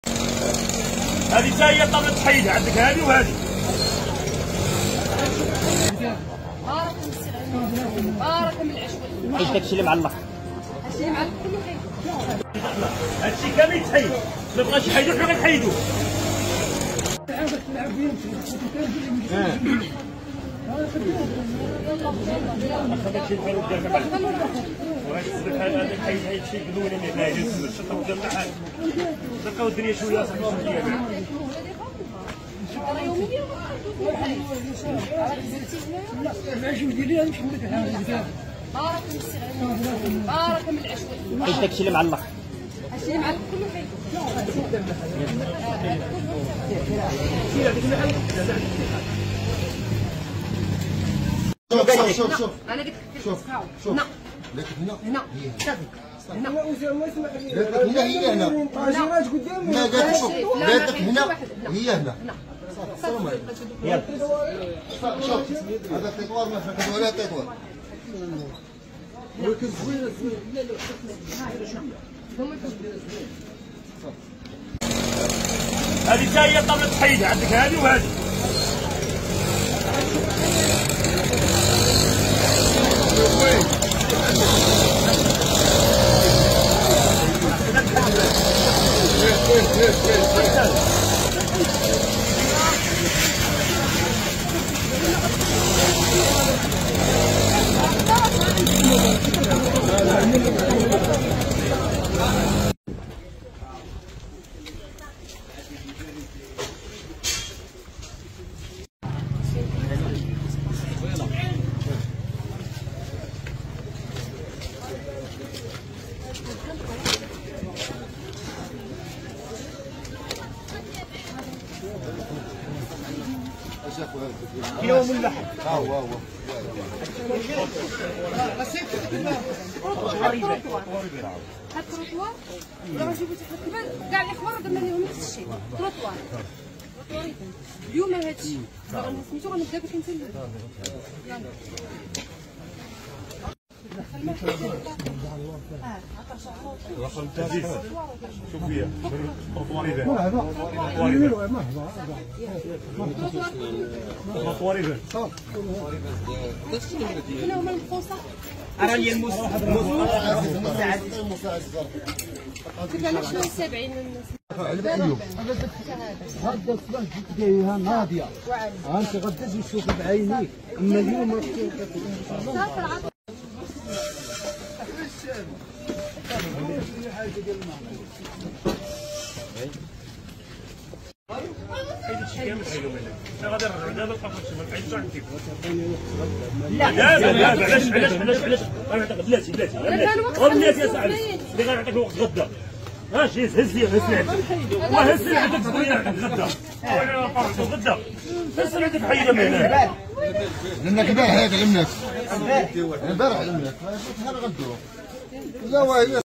🔴 برافو ✅️✔️ القائدة مازال مستمرة في حملة تحرير الملك العمومي بالجوطية بصرامة 💯🚨
س